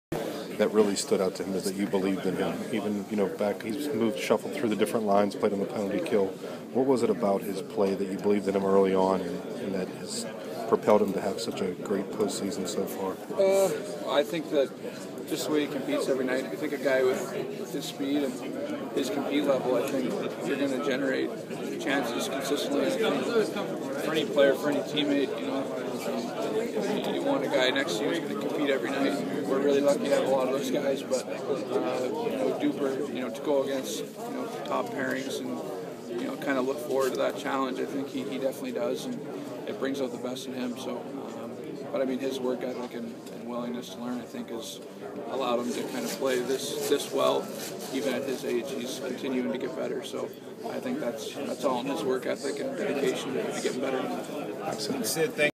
Crosby talks Dupuis
Penguins captain Sidney Crosby talks about what makes him believe in linemate Pascal Dupuis